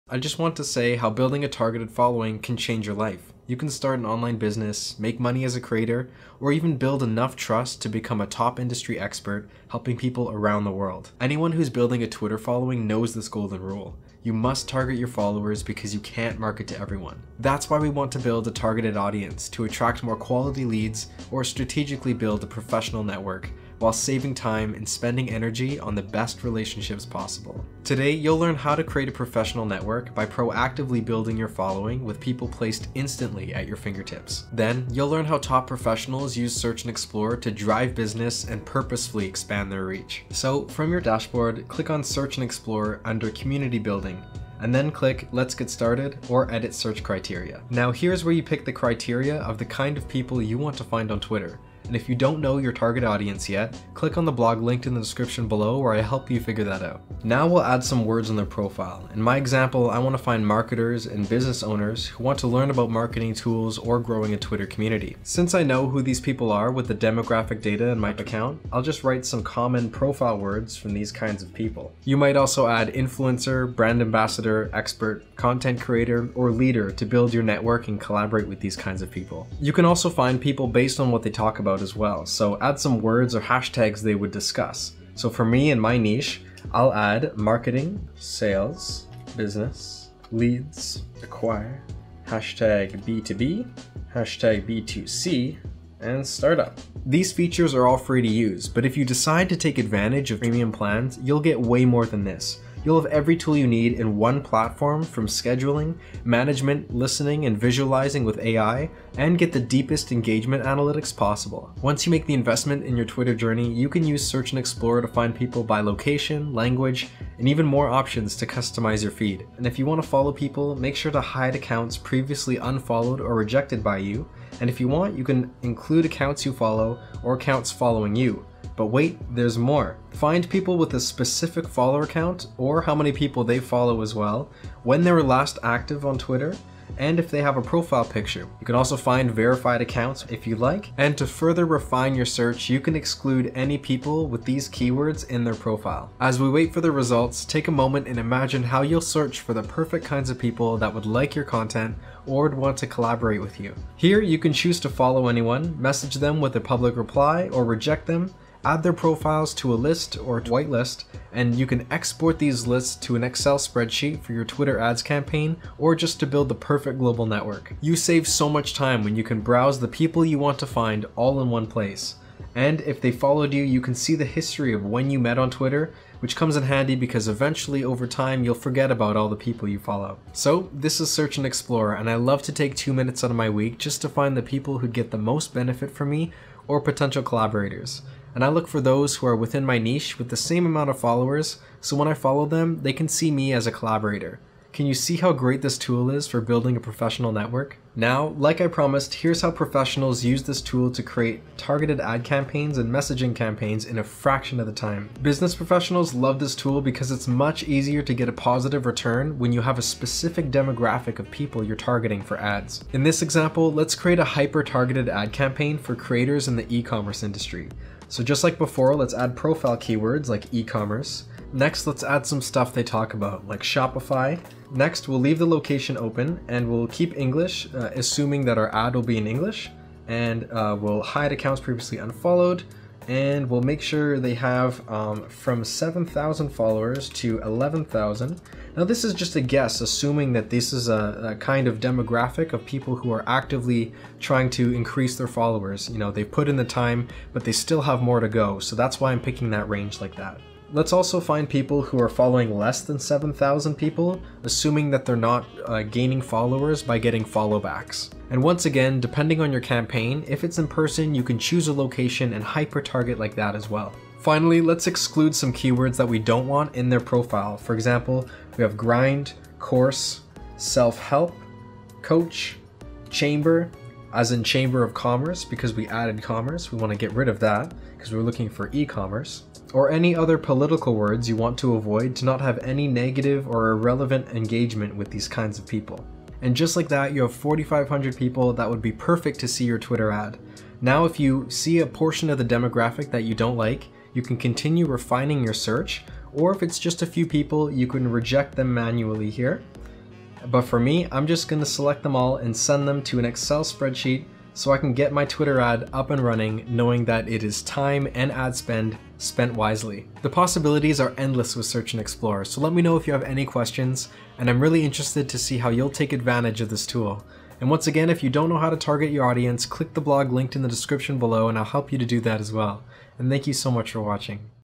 Search Bios, Words, Demographics to Find Twitter Accounts on X
Build a targeted professional network on X (Twitter) in seconds or find the perfect people to follow that will gain value from your content with Search & Explore, a FREE tool for your Twitter growth journey! Then, when you start making money on Twitter and investing in your growth journey, you can rely on this tool to proactively attract more business from the right people worldwide!

Find people who say certain things or hashtags or have specific keywords in their profile bio. Then, when you're ready to take it to the next level and access tons of other management, visualization, analytics and smart segmented scheduling tools, you'll unlock Search & Explore by location, language, follower count and more!

It's easy to find the best influencers with a specific follower size who live wherever and talk about e-commerce, for example. This way, you can create a global network of high-quality people for your business or collaborations!

📈Here's the blog guide to define your target audience & take your account from follower analysis to online business:

for free!

Jump to a section:
00:00 The Golden Rule of Growth
00:34 What You'll Learn
01:04 Define Who You Want to Find
02:35 Exploring Your Targeted Users
03:27 Find Leads & Collabs for Business
04:55 Applying Your Results